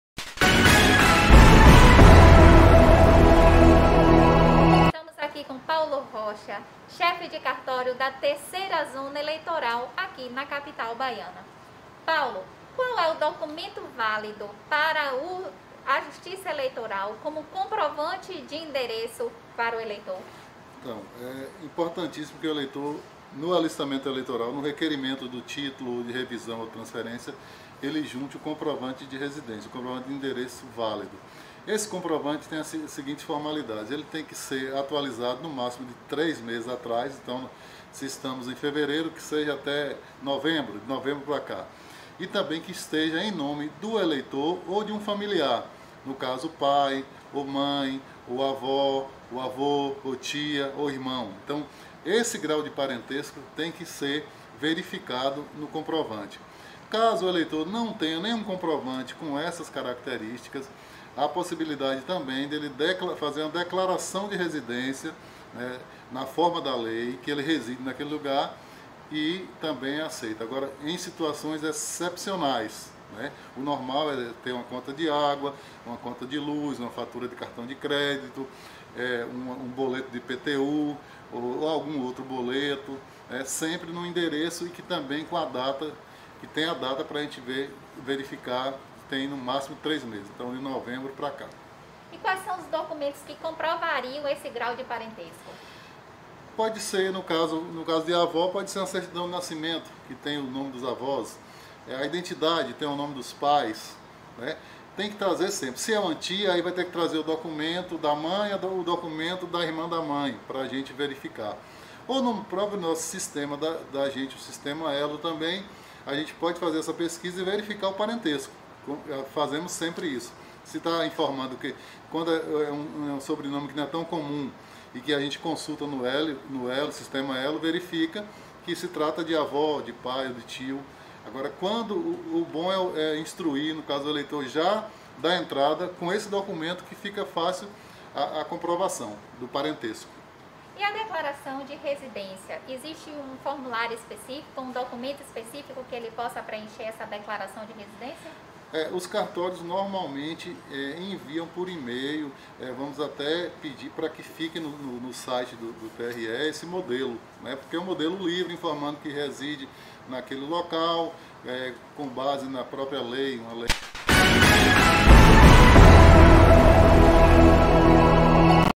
Ep. 24 - Comprovante de residência para a Justiça Eleitoral
Para fazer inscrição ou transferência de local de votação, há a necessidade de comprovar a residência. Para tanto, é necessário documentos em nome do próprio eleitor ou de parente. Boleto de água, luz, telefone, extrato de cartão de crédito etc, são alguns dos comprovantes válidos. No caso da ausência deles, a Justiça Eleitoral aceita uma declaração de endereço de residència. Quer saber como pode ser feito? É o que Paulo Rocha, chefe de cartório da 3* Zona Eleitoral, explica nessa 10ª edição do Cidadão em Pauta

Aperta o play!